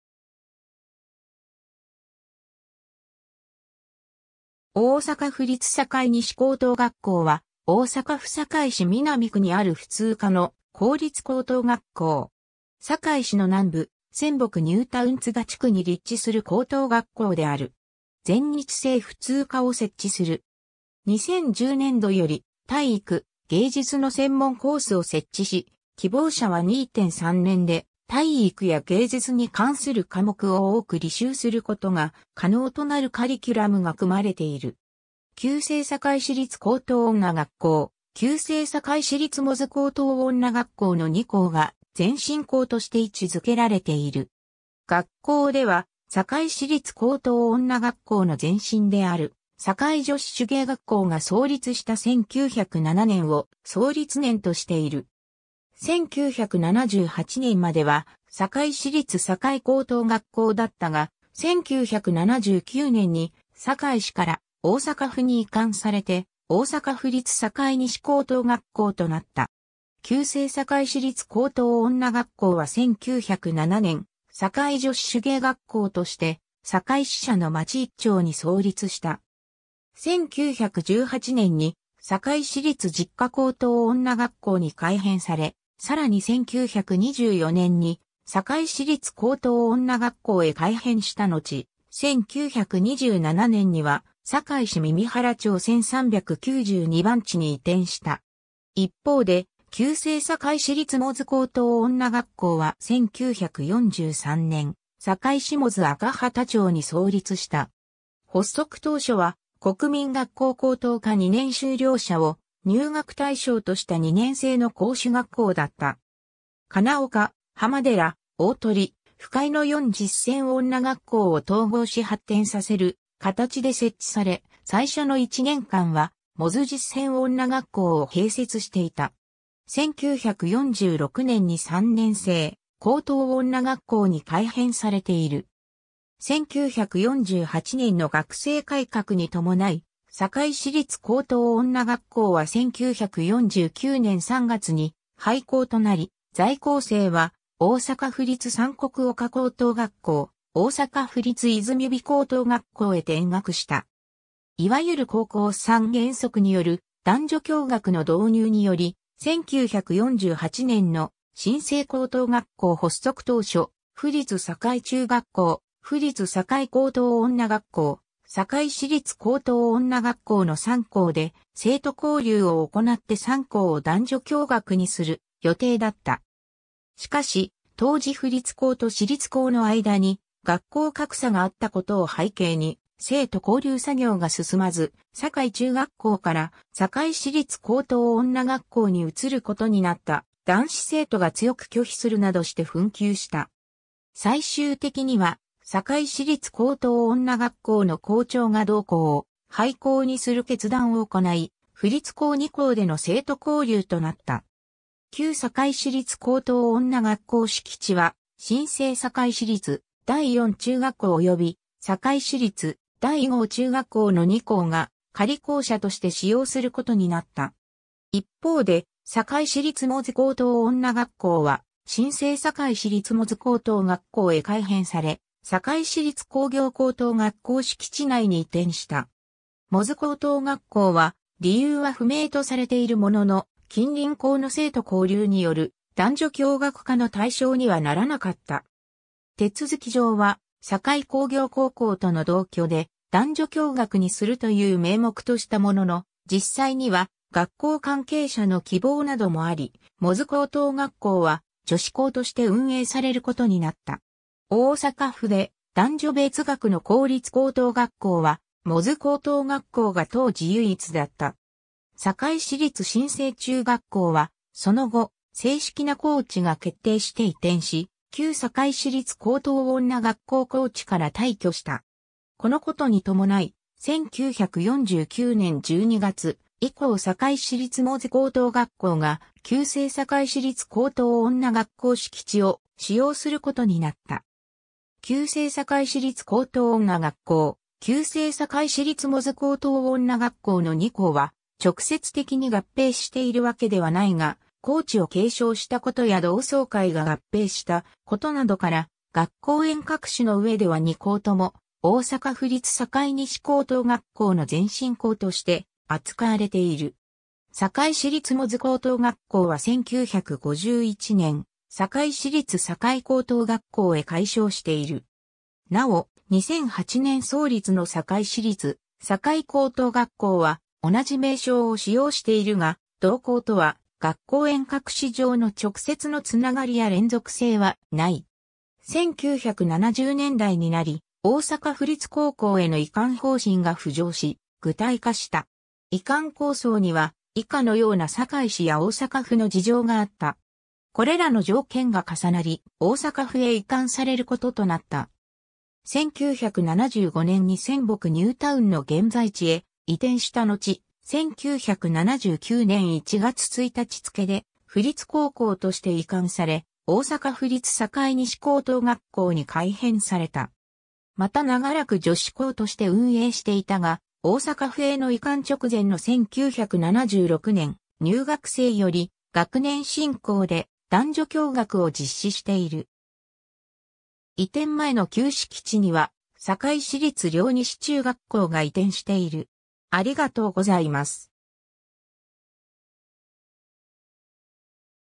大阪府立堺西高等学校
大阪府立堺西高等学校（おおさかふりつ さかいにしこうとうがっこう）は、大阪府堺市南区にある普通科の公立高等学校。
 堺市の南部、泉北ニュータウン栂地区に立地する高等学校である。
全日制普通科を設置する。
2010年度より体育・芸術の専門コースを設置し、希望者は2・3年で体育や芸術（音楽・美術・書道）に関する科目を多く履修することが可能となるカリキュラムが組まれている。
 旧制堺市立高等女学校（1907年創立）・旧制堺市立百舌鳥高等女学校（1943年創立）の2校が前身校として位置づけられている。
学校では、堺市立高等女学校の前身である堺女子手芸学校が創立した1907年を創立年としている。
1978年までは堺市立堺高等学校だったが、1979年に堺市から大阪府に移管されて大阪府立堺西高等学校となった。
 旧制堺市立高等女学校は1907年、堺女子手芸学校として堺市車之町1丁（現・堺区）に創立した。
1918年に堺市立実科高等女学校に改編され、さらに1924年に堺市立高等女学校へ改編したのち、1927年には堺市耳原町1392番地（現在の堺区大仙西町2丁）に移転した。
 一方で旧制堺市立百舌鳥高等女学校は1943年、堺市百舌鳥赤畑町（現・北区）に創立した。
発足当初は、国民学校高等科2年修了者を入学対象とした2年制の甲種学校だった。
金岡・浜寺・鳳・深井の4実践女学校を統合し発展させる形で設置され、最初の1年間は百舌鳥実践女学校を併設していた。
1946年に3年制高等女学校に改編されている。
 1948年の学制改革に伴い堺市立高等女学校は1949年3月に廃校となり、在校生は大阪府立三国丘高等学校・大阪府立泉陽高等学校へ転学した。
 いわゆる高校三原則による男女共学の導入により、1948年の新制高等学校発足当初、府立堺中学校（三国丘高校）・府立堺高等女学校（泉陽高校）・堺市立高等女学校の3校で生徒交流をおこなって3校を男女共学にする予定だった。
しかし当時府立校と市立校の間に学校格差があったことを背景に生徒交流作業が進まず、堺中学校から堺市立高等女学校に移ることになった男子生徒が強く拒否するなどして紛糾した。
最終的には堺市立高等女学校の校長が同校を廃校にする決断をおこない、府立校2校での生徒交流となった。
 旧堺市立高等女学校敷地は新制堺市立第四中学校（現在の堺市立大浜中学校）および堺市立第五中学校（現在の堺市立陵西中学校）の2校が仮校舎として使用することになった。
 一方で堺市立百舌鳥高等女学校は新制堺市立百舌鳥高等学校へ改編され、堺市立工業高等学校敷地内に移転した。
百舌鳥高等学校は、理由は不明とされているものの、近隣校の生徒交流による男女共学化の対象にはならなかった。
手続き上は堺工業高校との同居で男女共学にするという名目としたものの、実際には学校関係者の希望などもあり、百舌鳥高等学校は女子校として運営されることになった。
大阪府で男女別学の公立高等学校は、百舌鳥高等学校が当時唯一だった。
 堺市立新制中学校はその後、正式な校地が決定して移転し、旧堺市立高等女学校校地から退去した。
このことに伴い、1949年12月以降堺市立百舌鳥高等学校が旧制堺市立高等女学校敷地を使用することになった。
 旧制堺市立高等女学校・旧制堺市立百舌鳥高等女学校（百舌鳥高等学校）の2校は直接的に合併しているわけではないが、校地を継承したことや同窓会が合併したことなどから、学校沿革史の上では2校とも大阪府立堺西高等学校の前身校として扱われている。
 堺市立百舌鳥高等学校は1951年、堺市立堺高等学校へ改称している。
なお、2008年創立の堺市立堺高等学校は同じ名称を使用しているが、同校とは学校沿革史上の直接のつながりや連続性はない。
 1970年代になり、大阪府立高校への移管方針が浮上し具体化した。
移管構想には、以下のような堺市や大阪府の事情があった。
 これらの条件が重なり、大阪府へ移管されることとなった。
1975年に泉北ニュータウンの現在地へ移転したのち、1979年1月1日付で府立高校として移管され、大阪府立堺西高等学校に改編された。
また長らく女子校として運営していたが、大阪府への移管直前の1976年入学生より学年進行で男女共学を実施している。
 移転前の旧敷地には、堺市立陵西中学校が移転している。